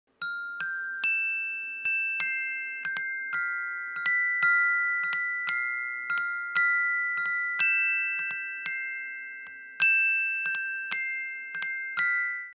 a non-music moment - rubber spiders on the ceiling and the fan that moves them
with a little xylophone music.

This has been a Barry James non-music moment.